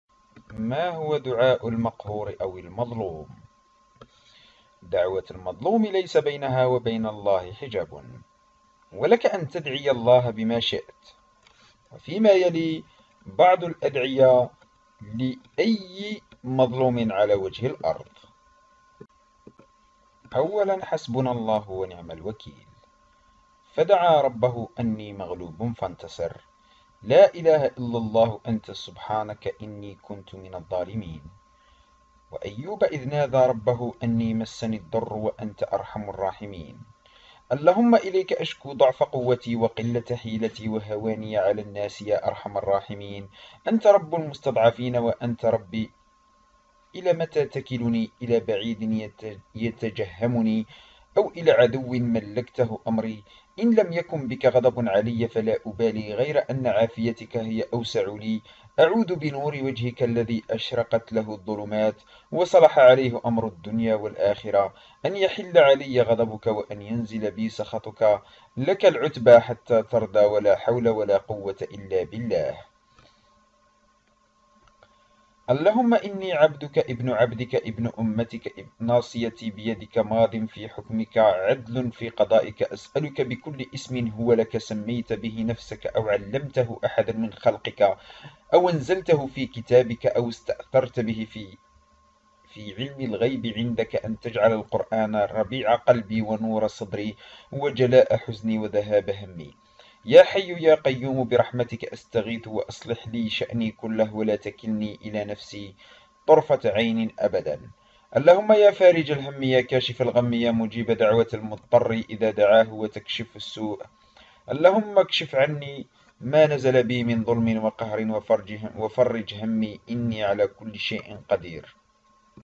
ما هو دعاء المقهور و المظلوم ؟
الله يجيب دعوة المضطر إذا دعاه، وأن دعوة المظلوم ليس بينها و بين الله حجاب، ولك أن تدعي الله بما شئت، وأضع بعض الأدعية لكِ في هذا الفيديو
God answers the supplication of the compelled if he calls him, and that the supplication of the oppressed is not between it and God a veil, and you can supplicate to God as you wish, and I put some supplications for you in this video